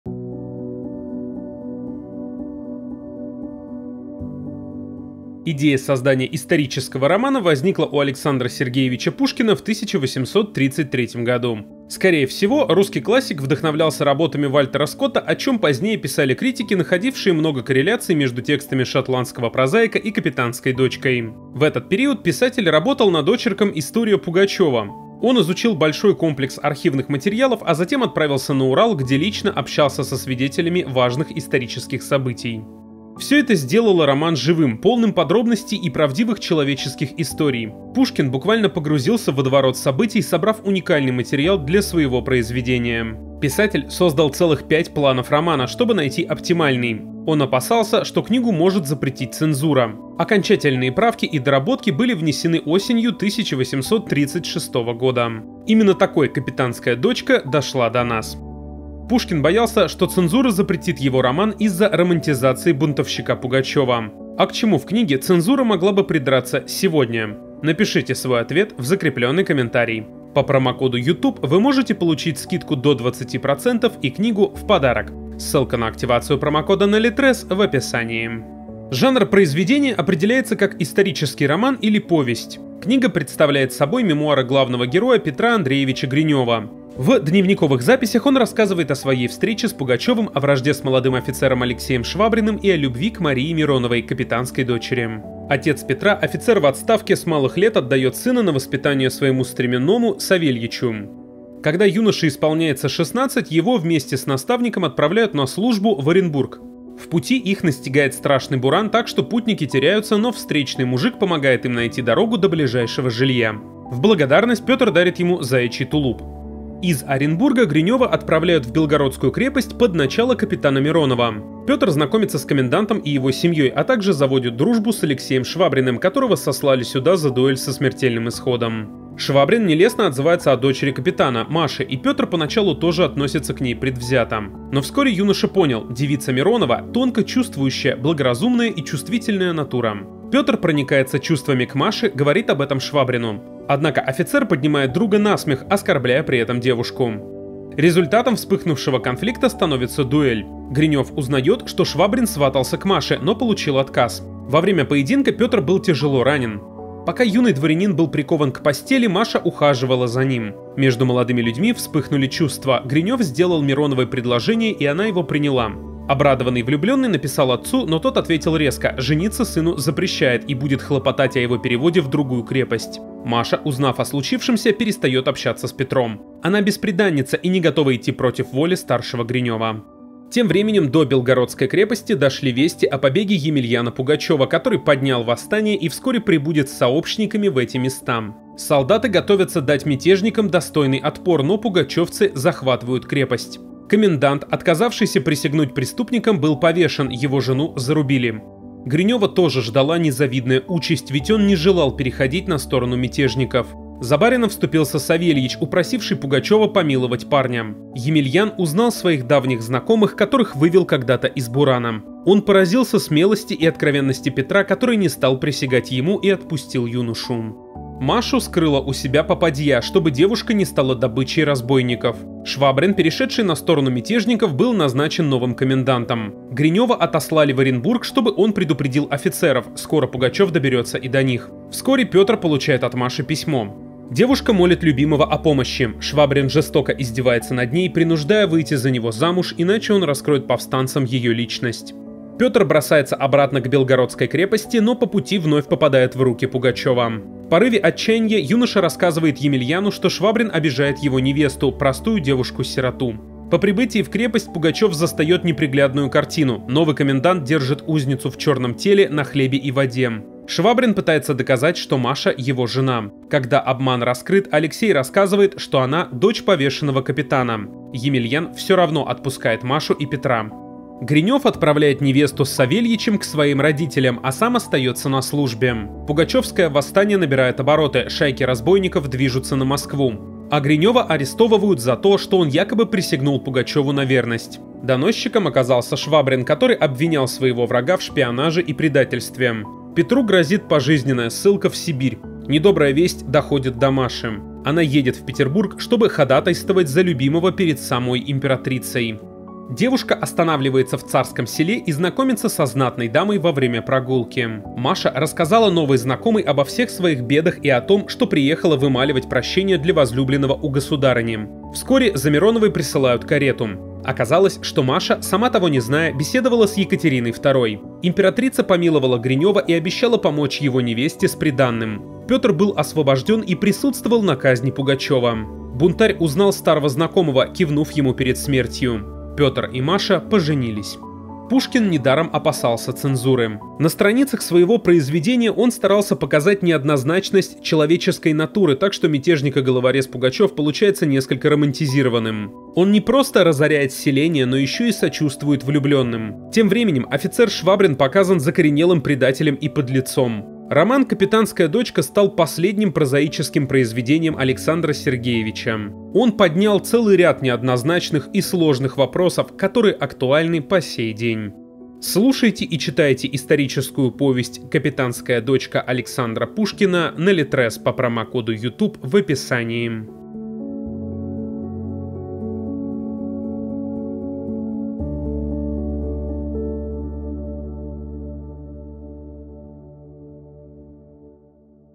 Капитанская дочка | Краткий пересказ
«Капитанская дочка» – роман А.С. Пушкина. Классик нарисовал яркую картину стихийного крестьянского восстания под предводительством Емельяна Пугачева.